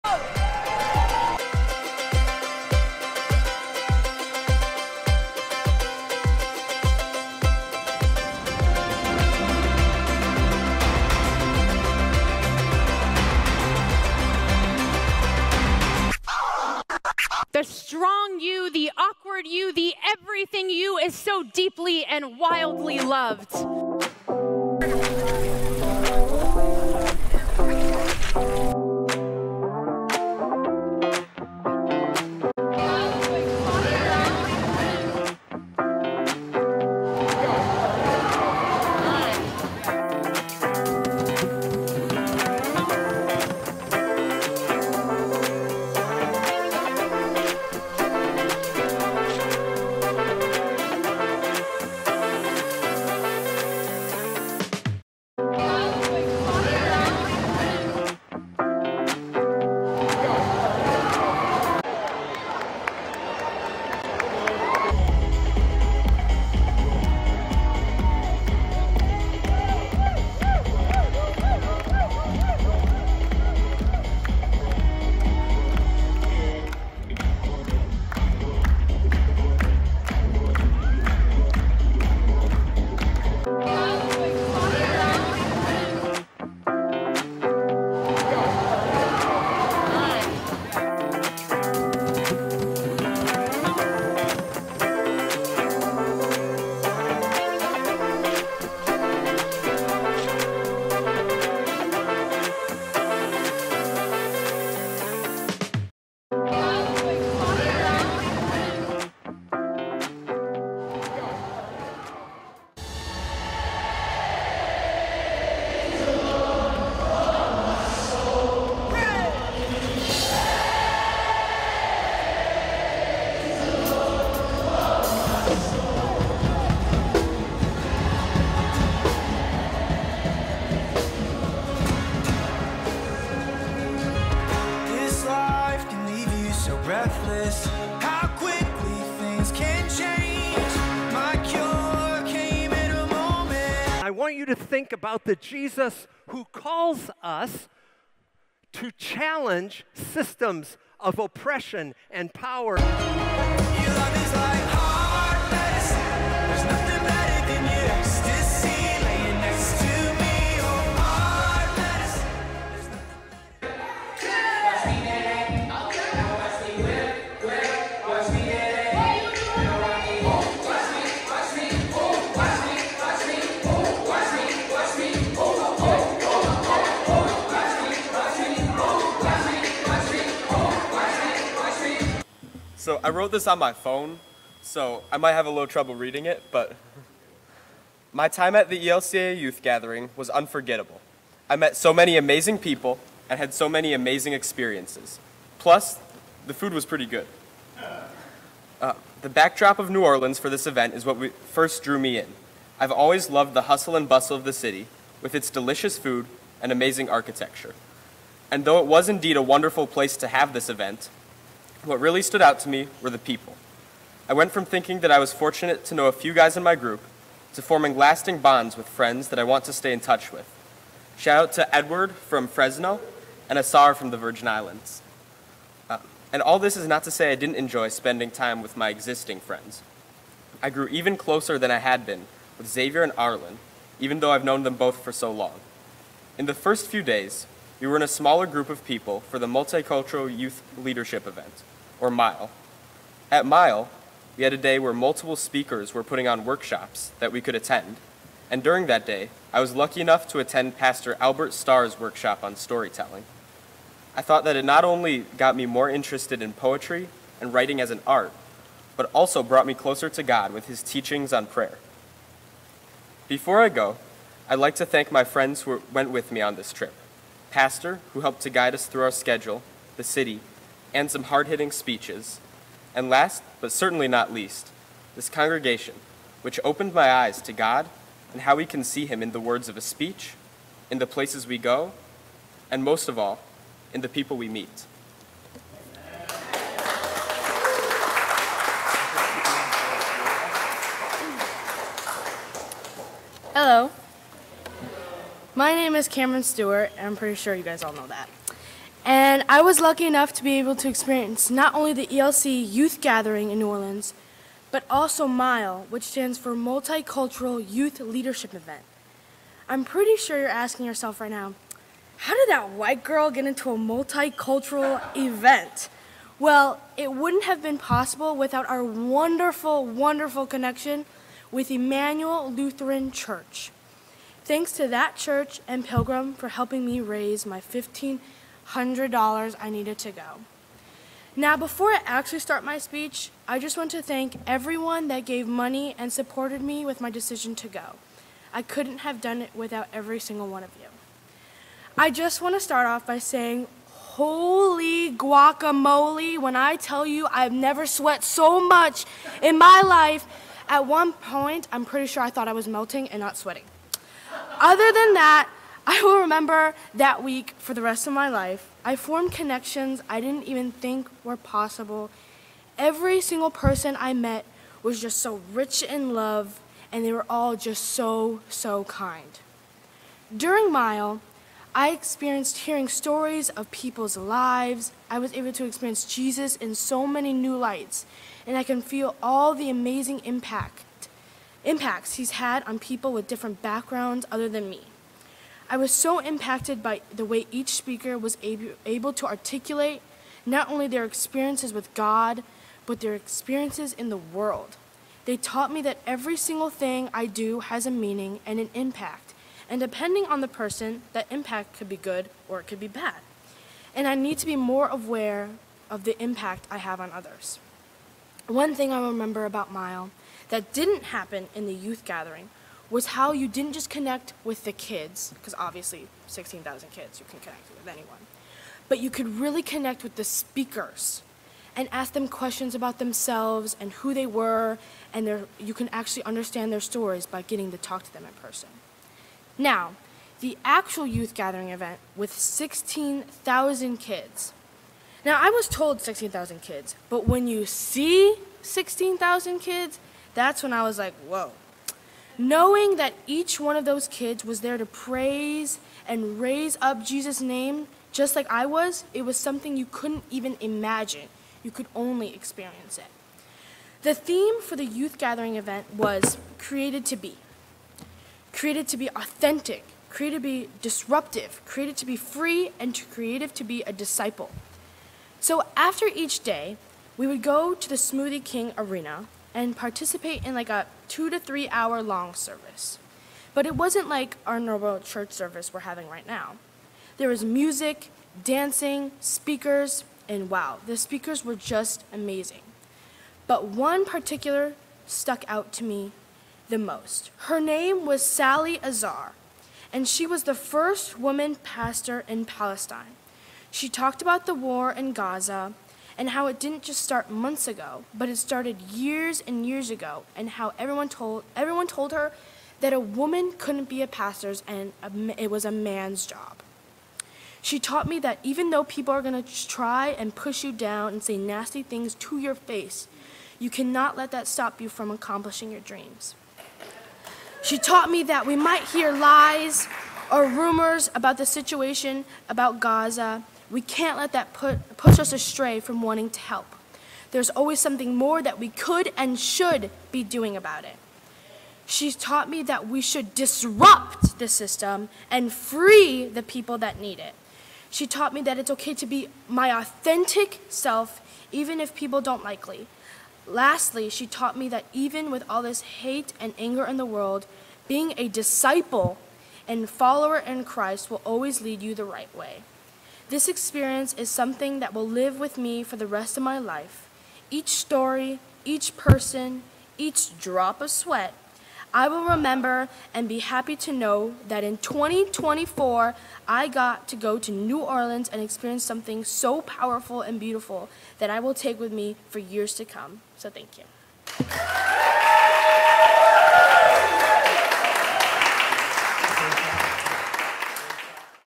ELCA Youth Gathering Testimonies Part II with Slideshow
Testimonies from New Orleans from Camryn and Clayton!  Well done! – The delegation of youth who participated in the ELCA Youth Gathering in New Orleans was EXEMPLARY. They showed up early to worship. They sat in the front. They stood up and praised God with their all! They connected with other youth from all over the country.  And they know they are loved and poised to make a difference! To enjoy the first two testimonies from Emanuel and Xavier,
Join us this coming Sunday, July 28th, at 9:30 a.m. to enjoy testimonies from Camryn and Clayton.  We praise God for our youth!